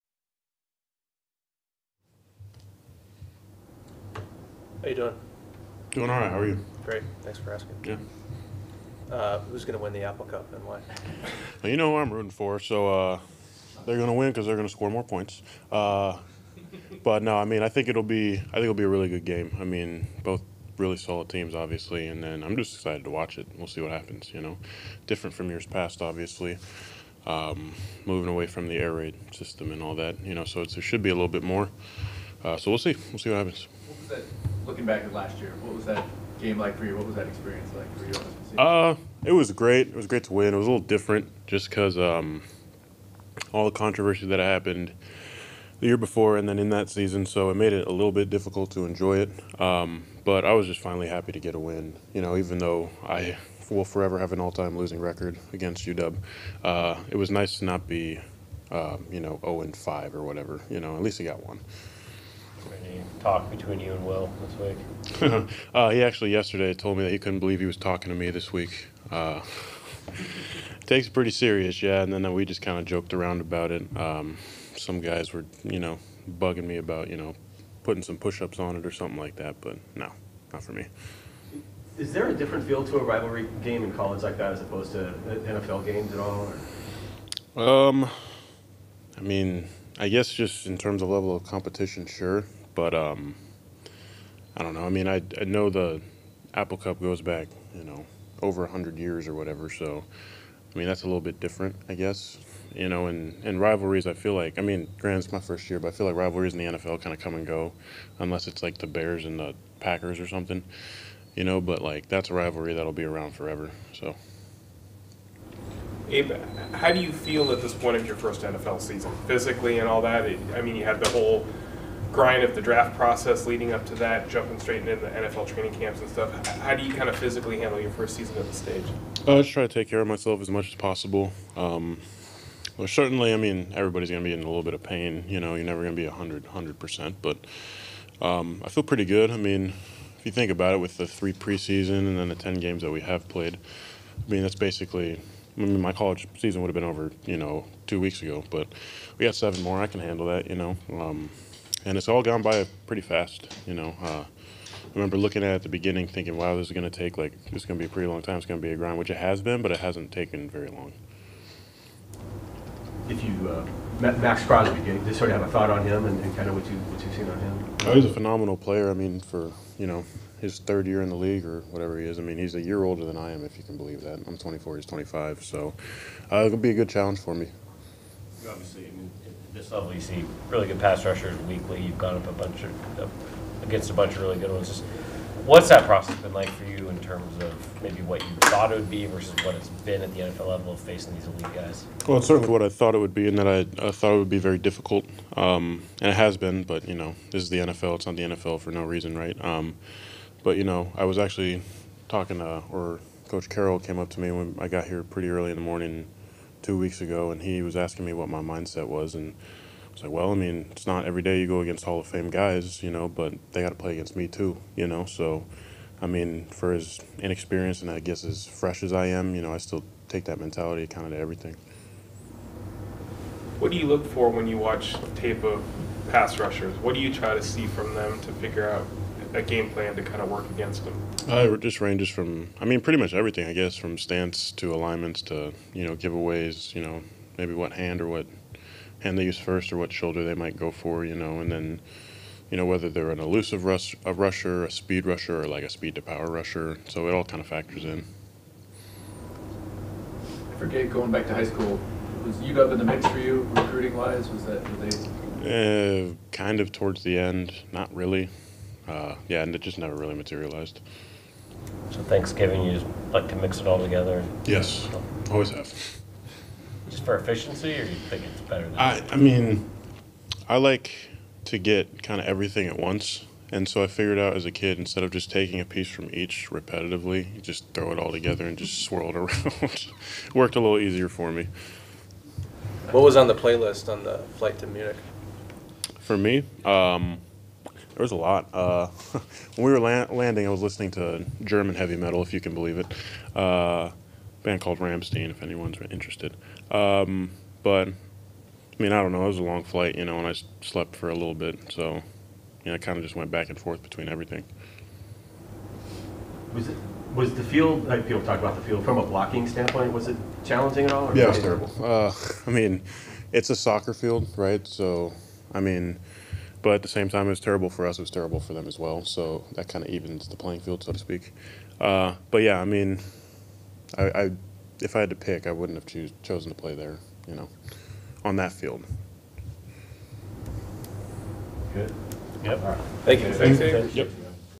Abraham Lucas Wednesday Press Conference - November 23
Abraham Lucas media prior to practice on Wednesday to talk about how his excitement for the Washington vs. Washington State game, the physically feels this far into his rookie season, and matching up against Raiders Maxx Crosby.